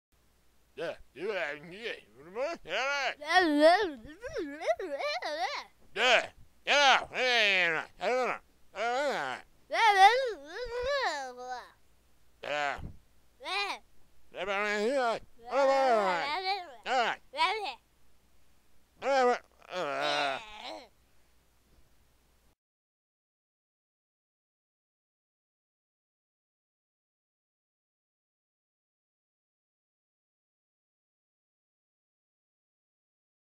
Genesis 11:7 (Confusion)
The confusion of various languages. From some curriculum I am working on for Pinecrest Community Church VBS in Parker, CO.